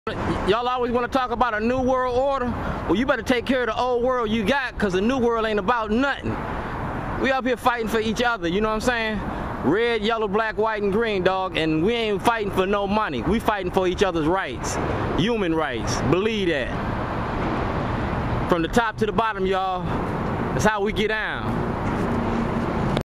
Shorty's City Stories We Back Standing up for the Homeless
We back protesting for the homeless. We don't need 1000 people to March..We got 100 people to pay attention to our videos and the ART of Protesting. We can do anything We put our minds too. Camera is a POWERFUL tool.